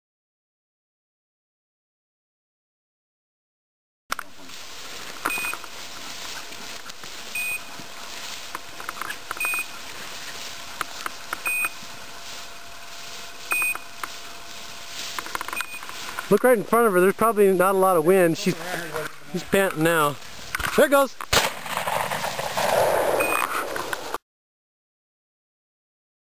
Mearns Quail Hunting Arizona
We headed out for a Mearns Quail Hunt December 12th and had a great time. In this clip we were chasing singles and Pistol pointed this one. We had 5 dogs out and one of them had a beeper on which you can hear. The bird held tight. Watch how Pistols tail starts to wag just before we notice the bird took off. Dogs are fast and perceptive!